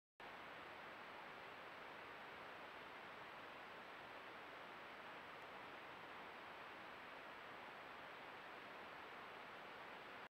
2021 Browning Spec Ops Elite HP4 Night Sample - Fat Racoon
Night Sample from the New 2021 Browning Spec Ops Elite HP4.